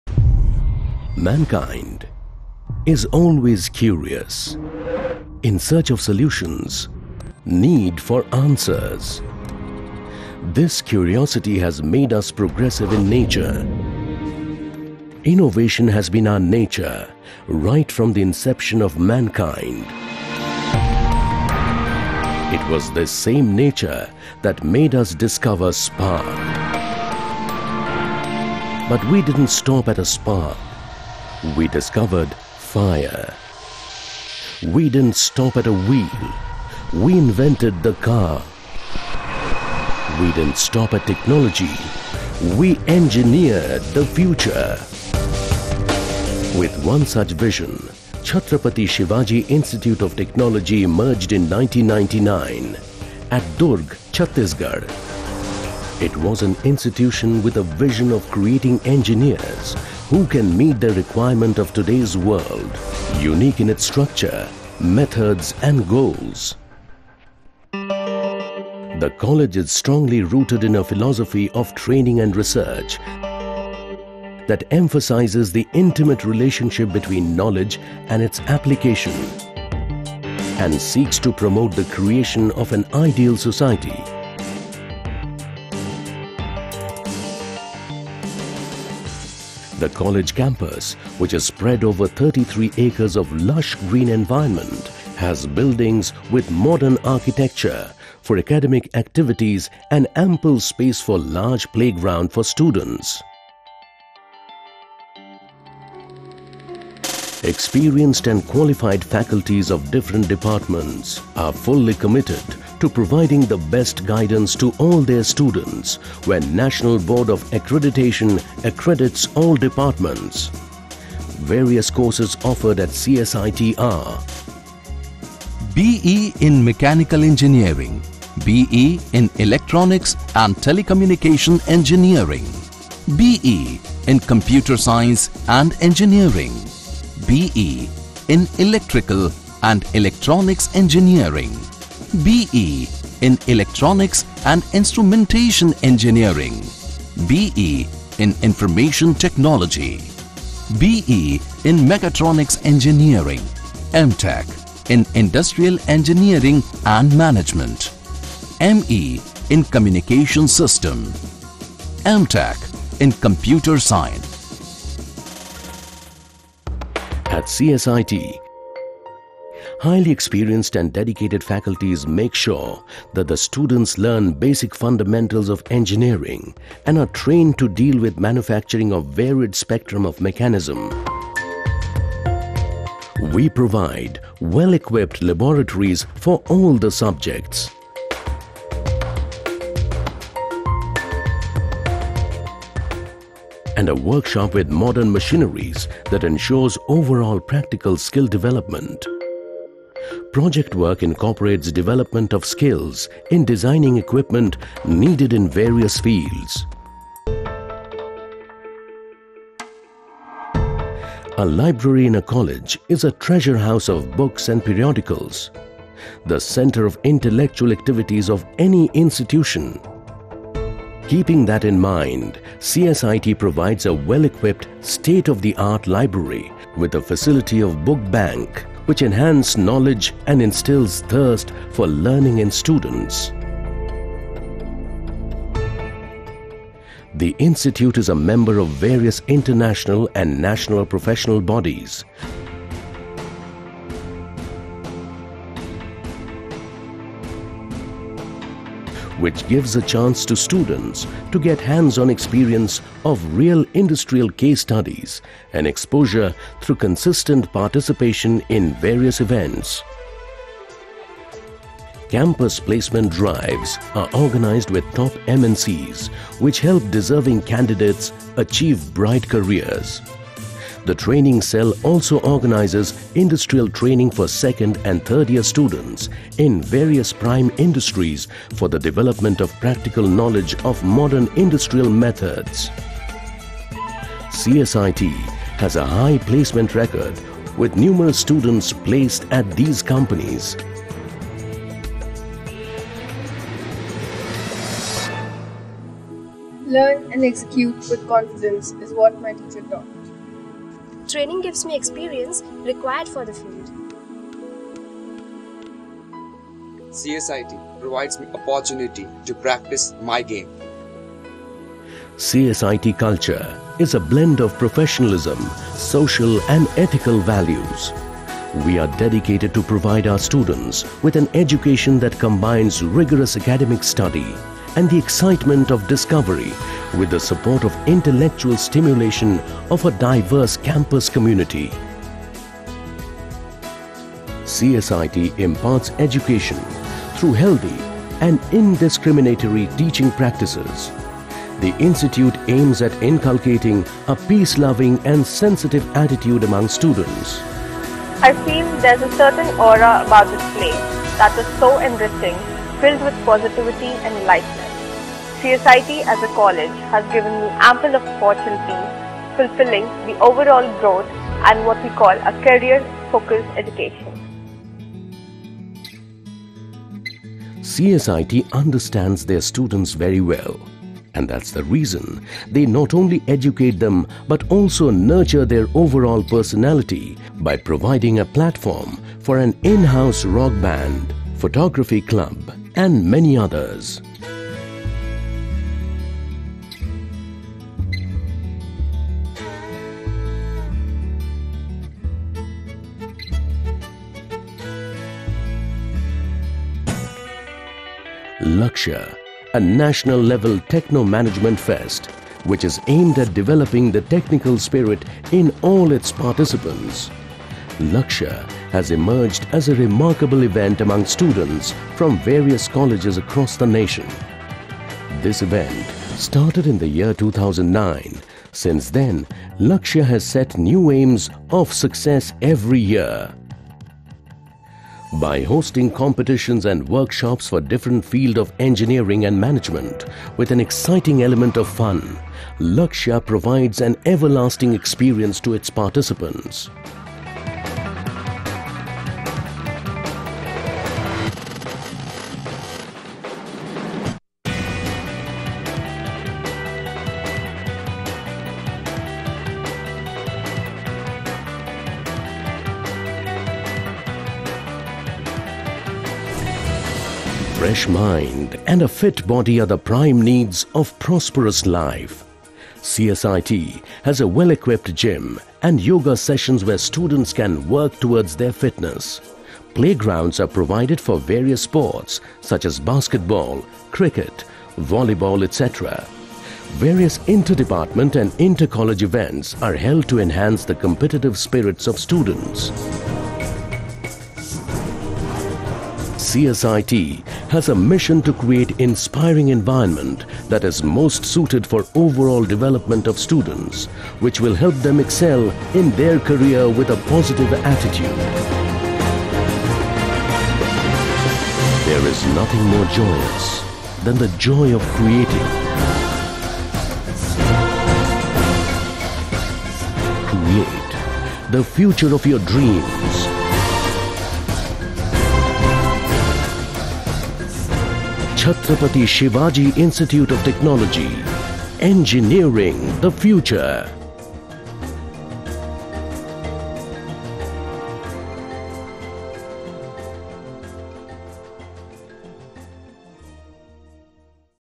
CSIT Durg - engineering the future (English)
CSIT is located on Durg – Balod, Dallirajhara Road at Shivaji Nagar near Kolihapuri, Dist. Durg (C.G.), 4Kms away from the Collectorate and 6 Kms from the Railway station, Durg. The grandeur of the location provides a unique environment for education in close proximity with nature which is far fetched from the hum-drum of the city. The CSIT campus has enrapturing & capitivating lush surroundings where silence is punctuated with birdsongs & gentle sway of trees. The total atmosphere adds to an aura of serenity & peace.

* Campus spread over 33 acres of land in eco friendly & lush green  green  environment.
* Campus floor area 2,00,000 sq.ft.( approx.) for academic activities.
* 40,000 sq.ft. floor area for laboratories.
* Central workshop with modern m/c & equipments.
* Canteen with modern amenities in an area of 5,000 sq.ft.
* Guest House with all luxuries in an area of 1500 sq.ft.
* Separate Girls/Boys hostel with 150 students capacity each in an area of 10,000     sq.ft each respectively.
* Staff quarters for Lecturers in a total area of 6,000 sq.ft. alongwith quarters for  HOD`s & Senior lecturers in a total area of 5,000 sq.ft.
* Amphitheatre in 3,000 sq.ft. area.
* Gymnasium with all modern equipments in 700 sq.ft. area.
* In Campus Bank.